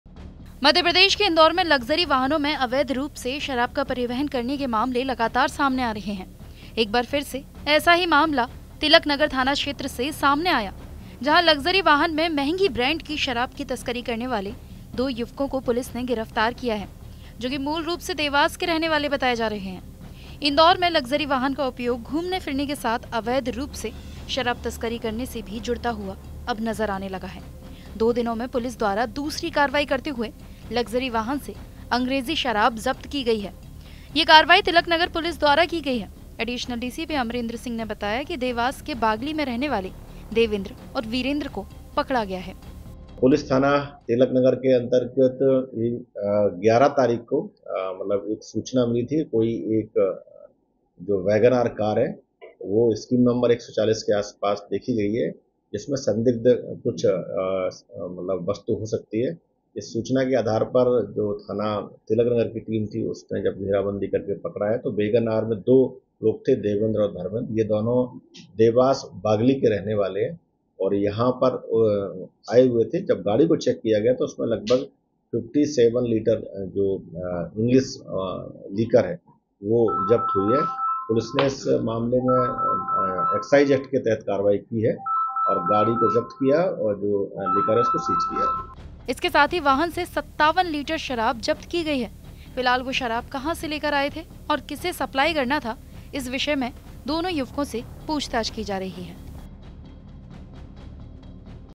Indore: लग्जरी कार से अवैध शराब की तस्करी, पुलिस ने की 57 लीटर अंग्रेजी शराब जब्त, 2 गिरफ्तार
मध्य प्रदेश के इंदौर में लग्जरी वाहनों में अवैध रूप से शराब का परिवहन करने के मामले लगातार सामने आ रहे हैं। एक बार फिर एक ऐसा ही मामला तहत तिलक नगर थाना क्षेत्र से सामने आया है। जहां लग्जरी वाहन में महंगी ब्रांड की शराब की तस्करी करने वाले दो युवकों को पुलिस ने गिरफ्तार किया है। जो की मूल रूप से देवास के रहने वाले बताए जा रहे है।

Cases of illegal transportation of liquor in luxury vehicles are constantly coming to light in Indore, Madhya Pradesh. Once again, a similar case has come to light from Tilak Nagar police station area. Where the police have arrested two youths who were smuggling expensive brand liquor in a luxury vehicle. They are said to be originally from Dewas.